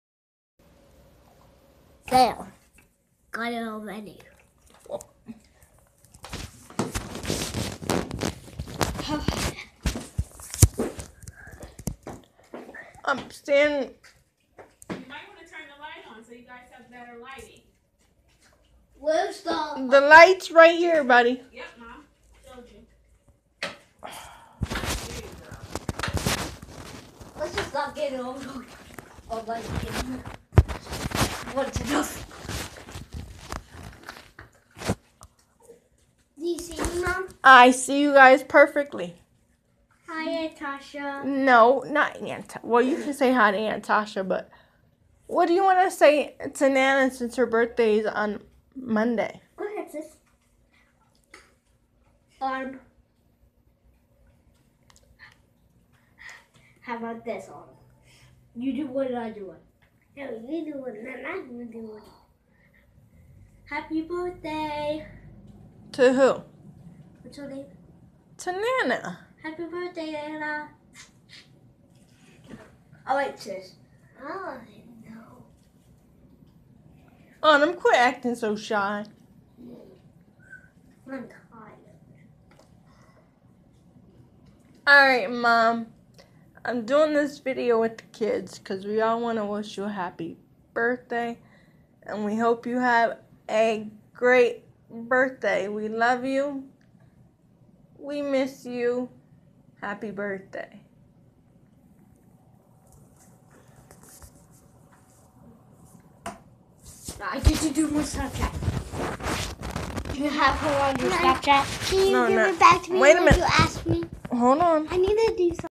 Happy birthday wishes to Nana from 💖 Autumn & Marcellus💖
Happy birthday wishes from your grandbabies